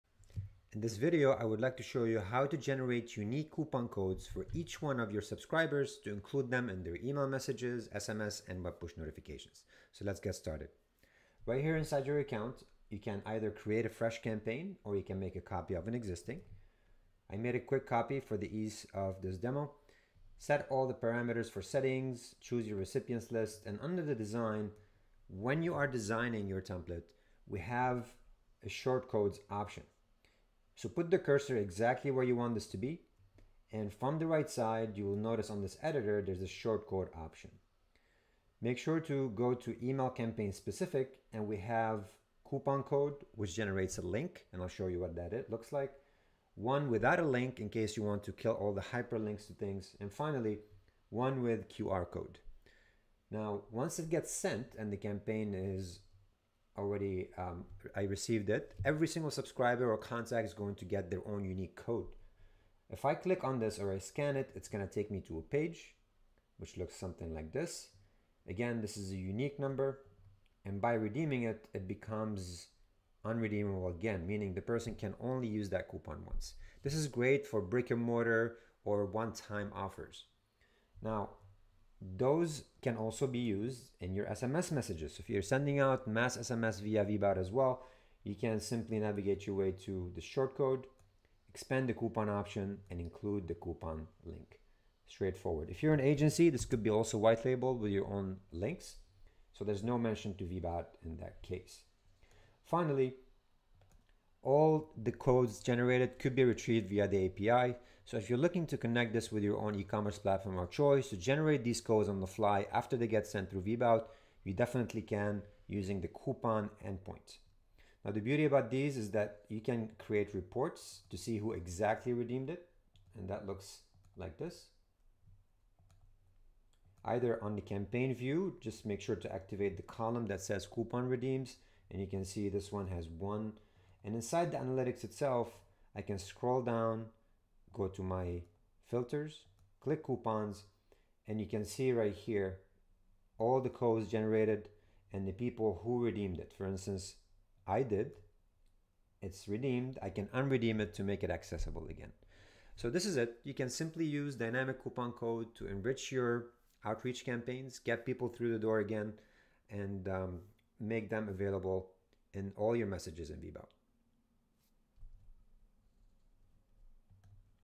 Create Coupon Campaigns
Coupon campaigns is a great way to get your leads to convert and your clients coming back for more. brickandmortar franchises and ecommerce businesses use them extensively in their strategy. Here is how you can create unique campaign codes in @VBOUT using a simple tag inside emails and sms campaigns.

- [00:00]( 🎯 Learn how to generate unique coupon codes for subscribers in email, SMS, and web push notifications within your @VBOUT account.
- [00:42]( 📝 Use the shortcode "coupon code" in the email campaign editor to generate coupon links or QR codes for each subscriber.
- [01:23]( 📱 Extend coupon usage to SMS messages by including coupon links in your Mass SMS via @VBOUT.
- [01:51]( 🌐 Connect with your e-commerce platform through the API to generate these codes on the fly after sending them via @VBOUT.
- [02:27]( 📊 Track coupon redemptions and manage them within the @VBOUT platform, allowing you to analyze campaign performance and engagement.